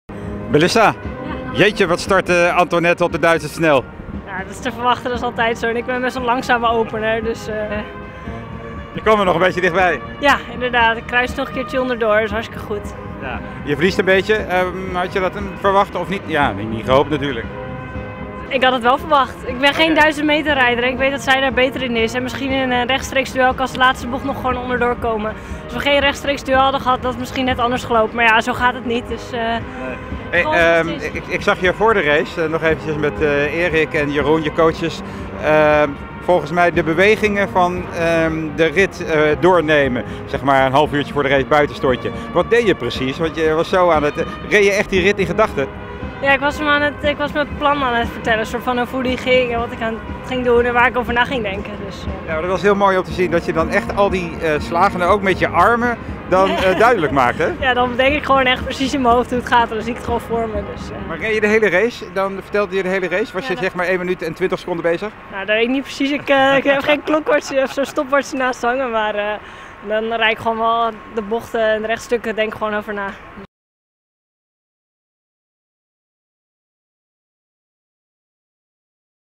Het 'losse armen'-raceplan van Melissa Wijfje
Melissa Wijfje werd vrijdag wereldkampioene op de 1500 meter voor junioren. Het leek er tijdens de race op dat ze in een sprint naar de finish reed en dat bleek ook precies de bedoeling te zijn.